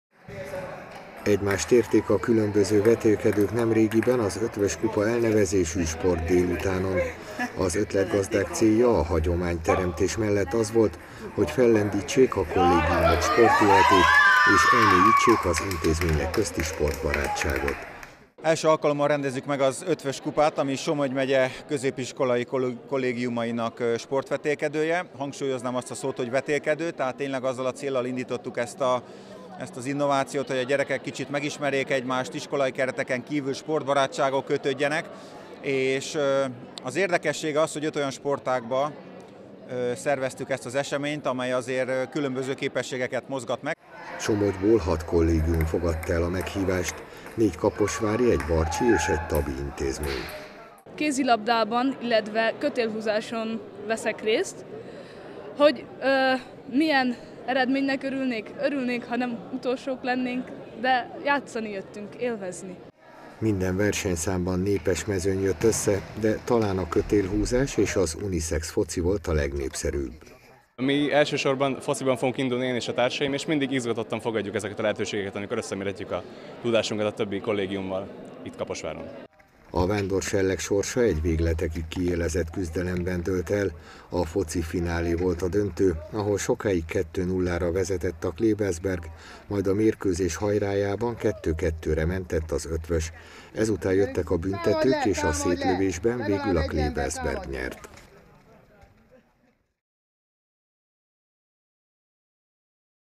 Somogyi kollégiumok vetélkedője
Az Eötvös Kupát idén először írták ki hagyományteremtő szándékkal. A résztvevőket elnézve, biztosan lesz még folytatás.